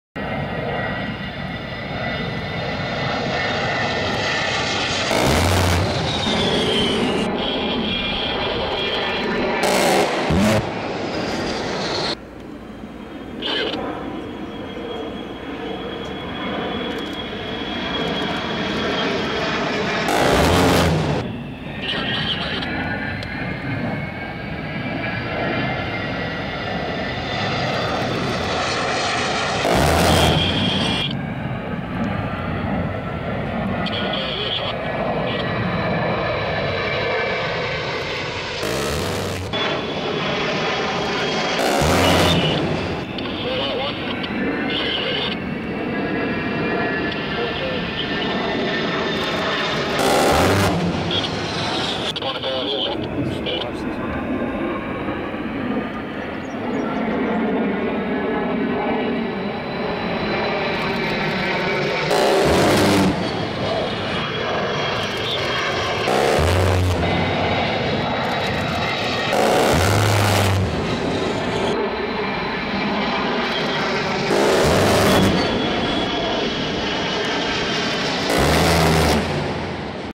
A-10C Strafing Runs
47th Fighter Squadron A-10s strafing at Claiborne Bombing & Gunnery Range in Alexandria, LA